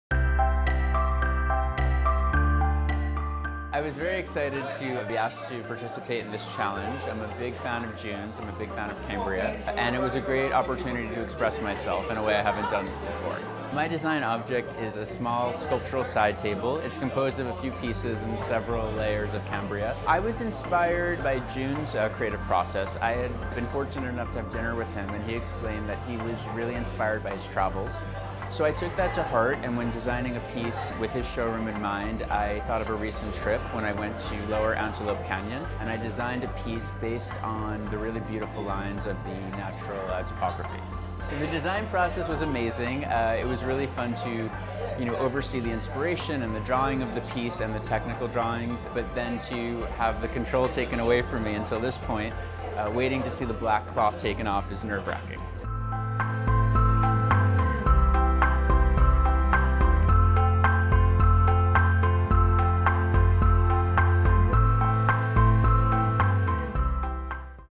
Bennett Leifer: The Kristina Table Collaboration
Bennett Leifer's signature Kristina Table was inspired by a recent trip to Utah and Arizona with his friend Kristina, where the pair was awestruck by the beauty of the landscape. Recalling the soft sculptural and natural lines of Lower Antelope Canyon, Leifer worked with the Cambria fabrication team to create an occasional table featuring a base composed of two separate pieces as an homage to the ever-evolving form of the canyon and nature itself. Composed of six layers of different Cambria pieces wrapped in White Cliff™ and topped with glass, this stunning table invites one to look “into” the depth of the piece.